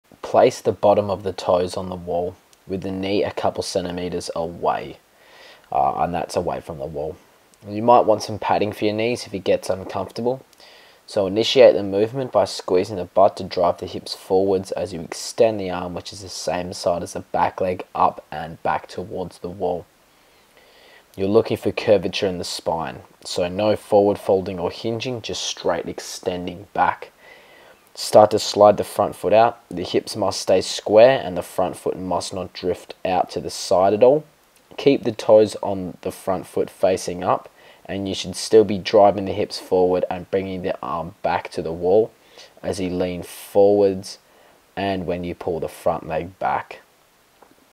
Front split slides
- Place the bottom of the toes on the wall, with the knee a couple of cm’s away from the wall.
- Initiate the movement by squeezing the butt to drive the hips forwards, as you extend the arm which is the same side as the back leg up and back towards the wall.
- You’re looking for curvature in the spine, no hip hinging.
- Start to slide the front foot out, the hips must stay square and the front foot must not drift out to the side at all.
 - Keep the toes on the front foot facing up.
- You should still be driving the hips forward and bringing the arm back to the wall as you lean forwards and when you pull the front leg back.